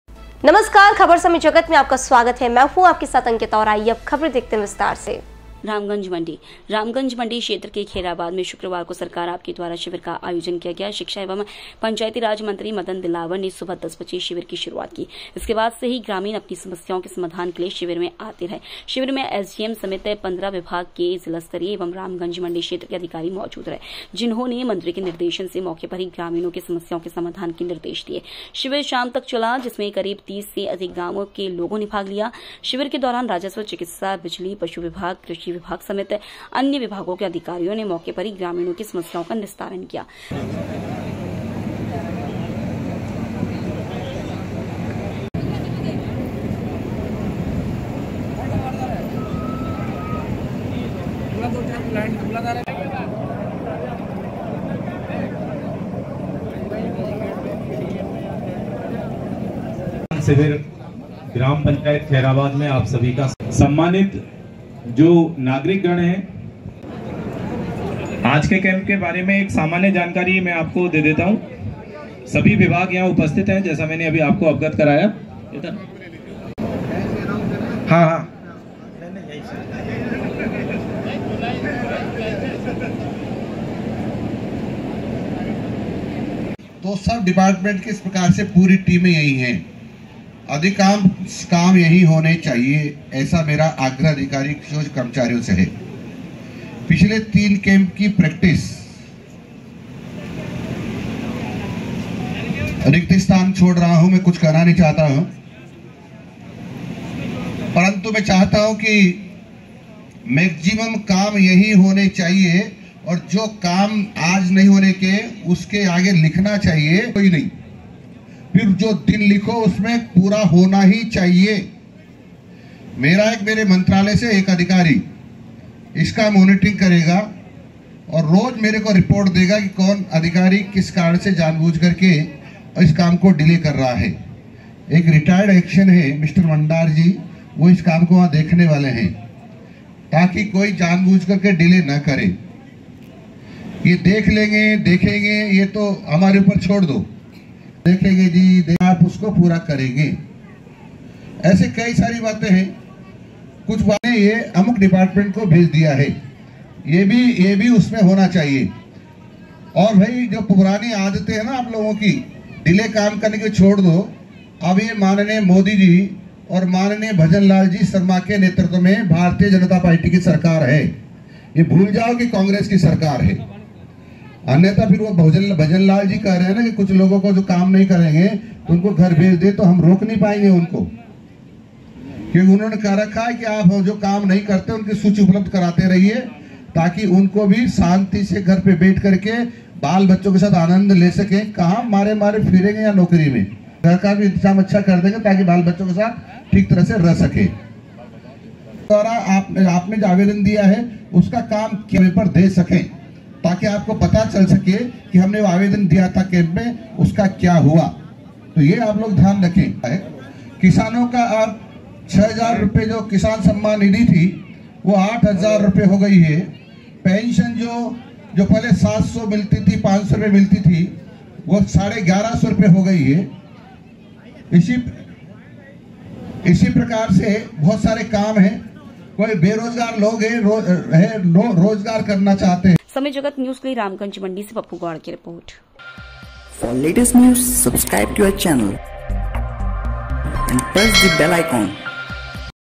रामगंजमंडी:- सरकार आपके द्वारः शिविर का आयोजन !mpnews
पप्पू गौड की रिपोर्ट